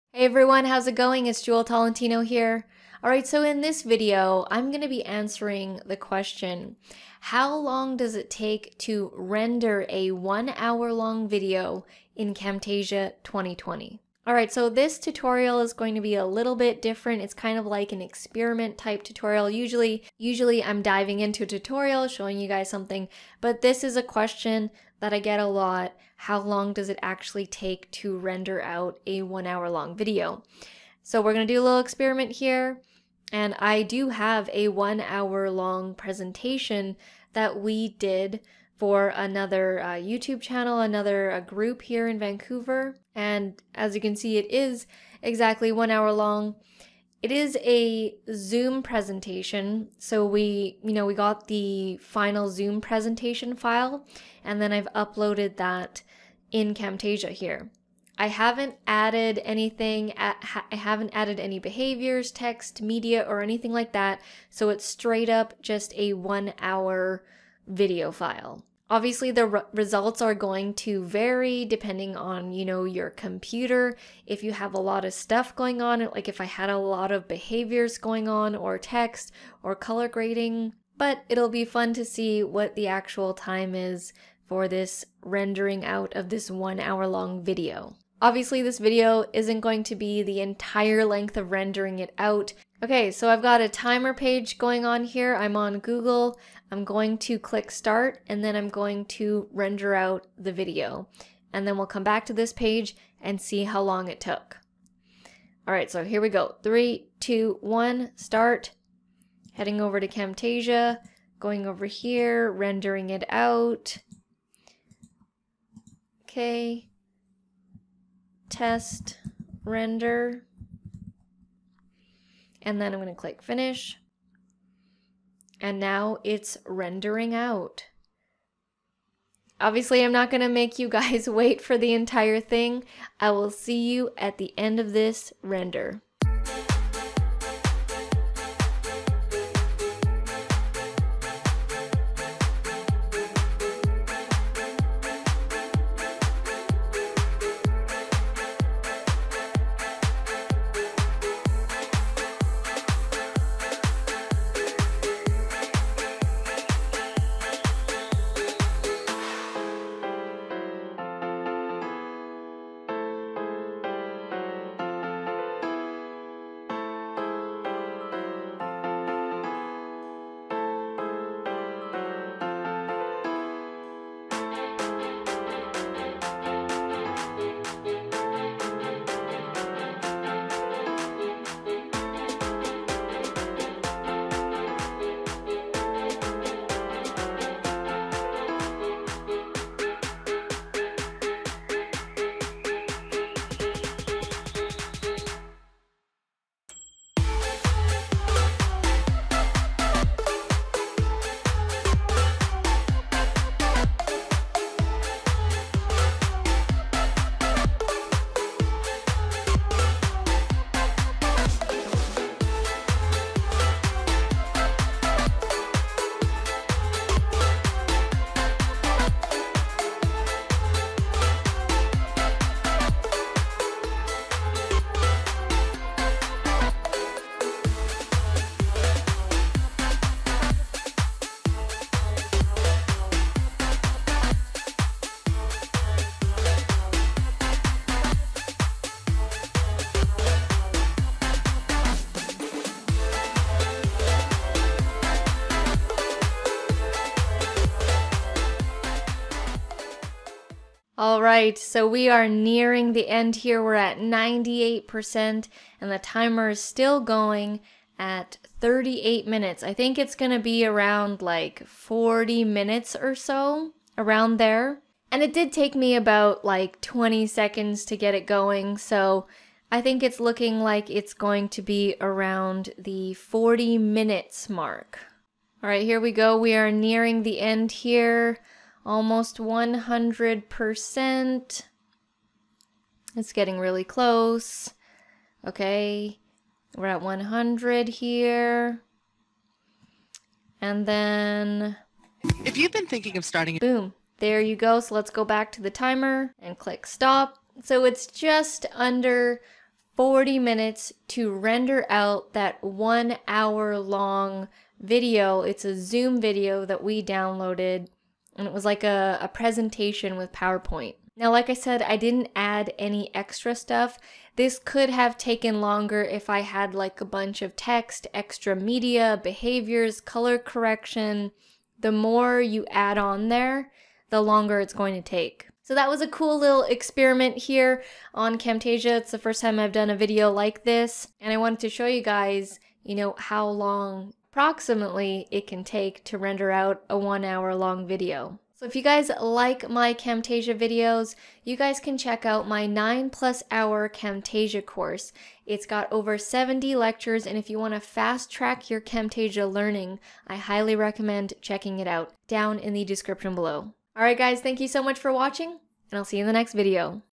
Camtasia 2020 How Long Does it Take to Render a 1hr Long Video? 🤷
Camtasia 2020 How Long Does it Take to Render //  This was just a fun experiment video I put together where I wanted to see how long it would take to render out a 1hr long zoom presentation.

🎥 ➤ OUR FILMING GEAR:

✦ Cameras:

1. DSLR Camera: Canon M50

2. Webcam Camera: Logitech c930e

✦ Microphones:

1. USB Mic: Fifine Technology

2. Lapel Mic: PowerDeWise